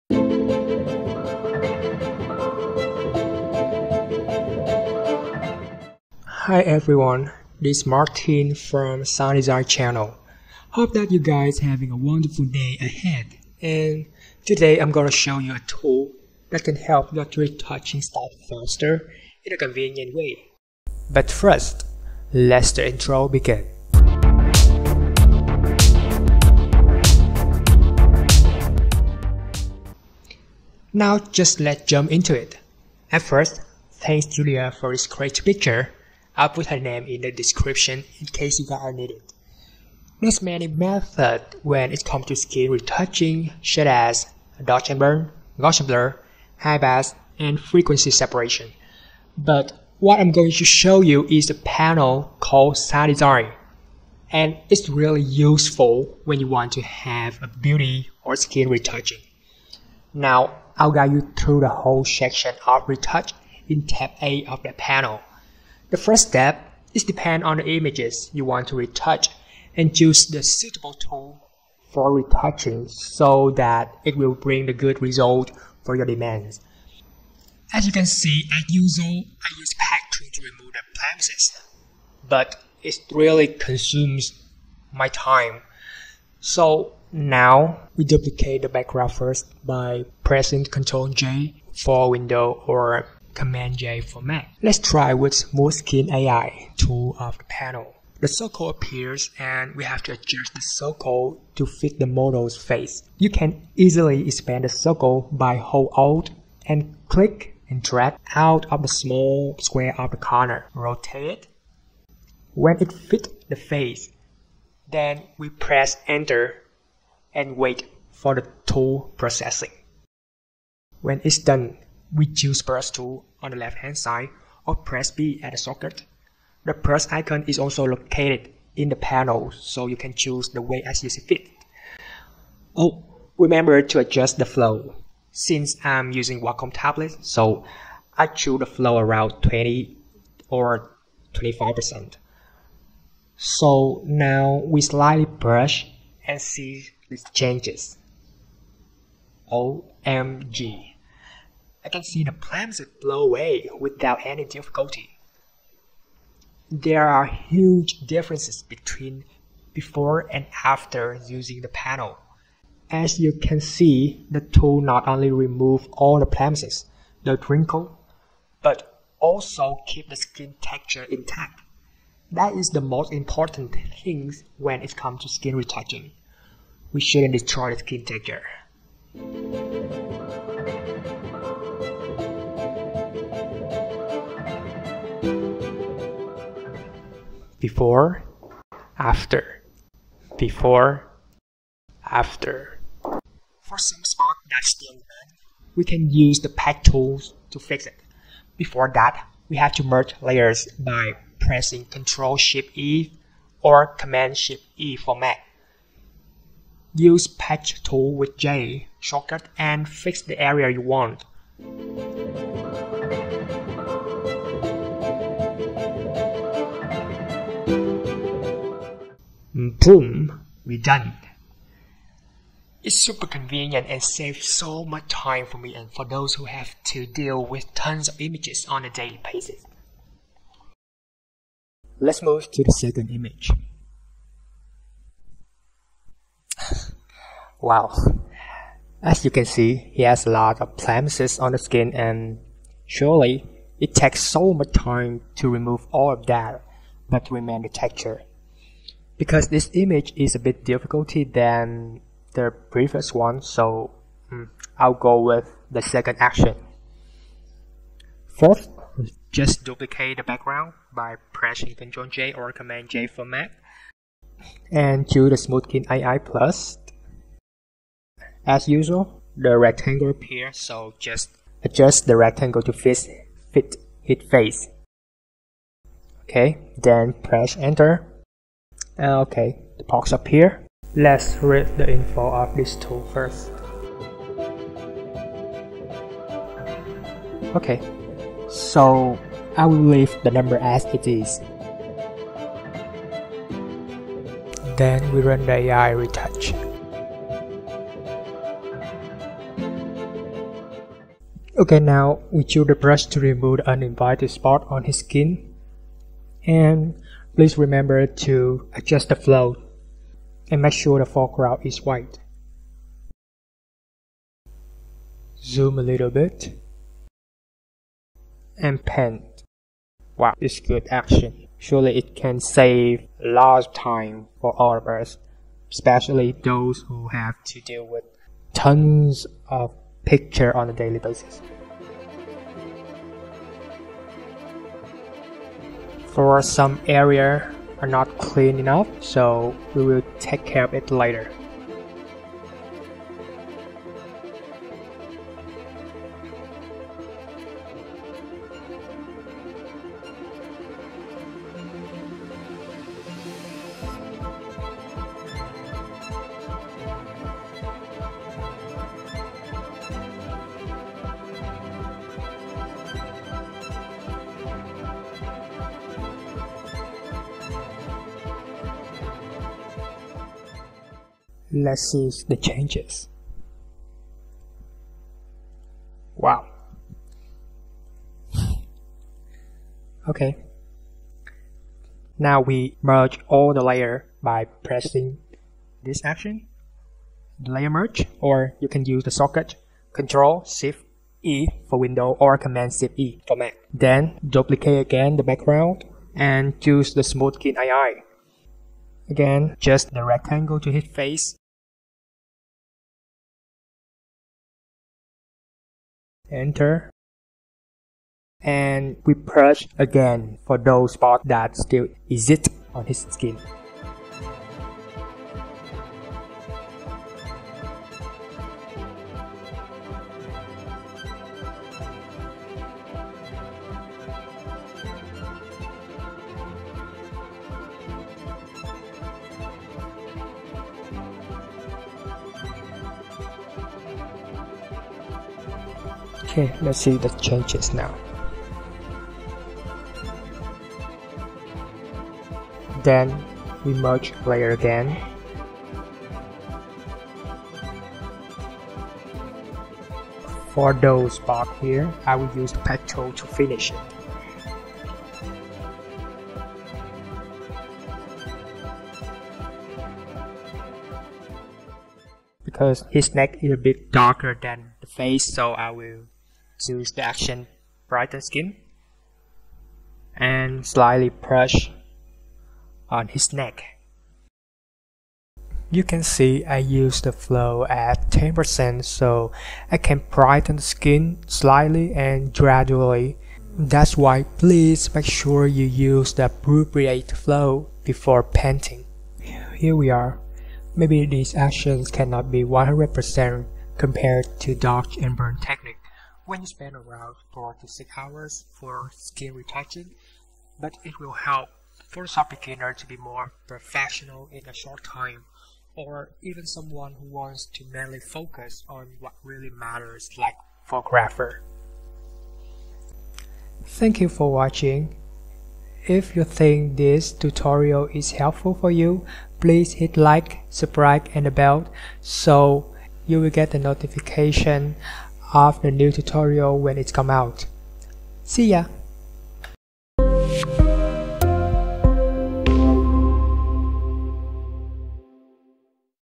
SADESIGN Retouching Smooth Skin AI
Sadesign Retouching Panel is a smart Photoshop extension designed by SADESIGN Academy to help you simplify and speed up your photo editing workflow. Using some extremely useful and advanced editing scripts, as well as the ability to add custom User Actions, the Sadesign Retouching Panel will also assist you in achieving precise quality editing results. and repeatable to help you complete the one-wing shot quickly.

Whether you are a freelance photo editor or a professional photographer, the Sadesign Retouching control panel will help increase your efficiency by implementing tedious repetitive parts of your routine and save you time. save countless hours of work in the long run.

Sadesign will help you with the free installation and dedicated support anytime, anywhere.